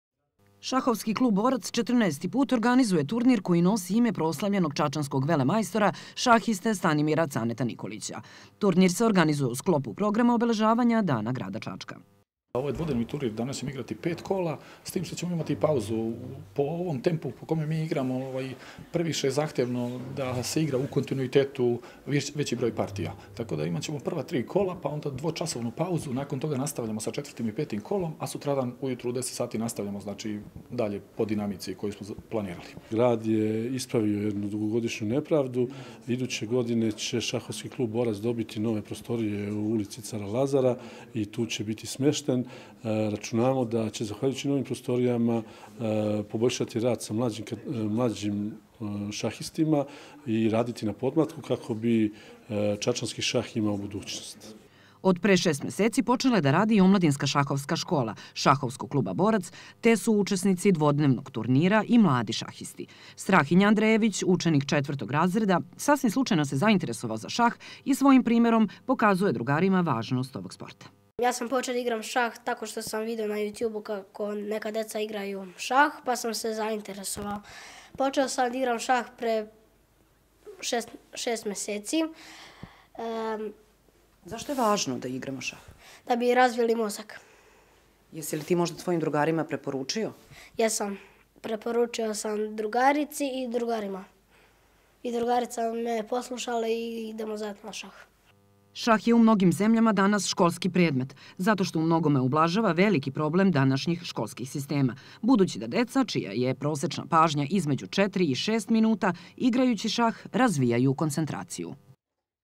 Vikend turnir u Čačku "Stanimir Cane Nikolić 2018"
U organizaciji šahovskog kluba Borac u hotelu Beograd danas je počeo dvodnevni 14.šahovski turnir Stanimir Cane Nikolić, u okviru obeležavanja dana grada Čačka. Pokrovitelj je grad, a učestvuje 40 šahista među kojima ima i najmlađi